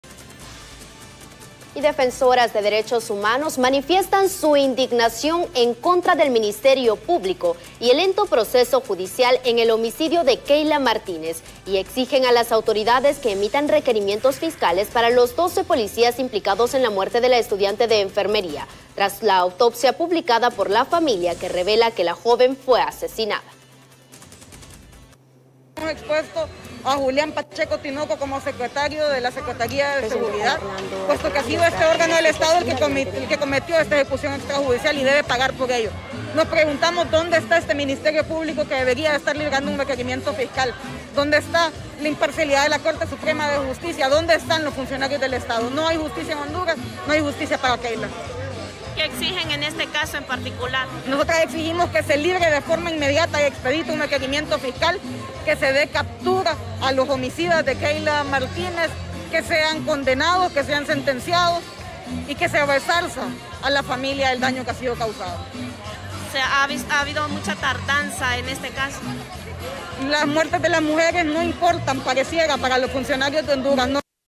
Exigen requerimientos fiscales en contra de los asesinos de Keyla Martínez
Defensoras de DD.HH manifiestan su indignación en contra del MP y el lento proceso judicial en el homicidio de Keila Martínez y exigen a las autoridades que emitan requerimientos fiscales para los 12 policías implicados en la muerte de la estudiante de enfermería, tras que la autopsia publicada por la familia, revelara que la joven fue asesinada.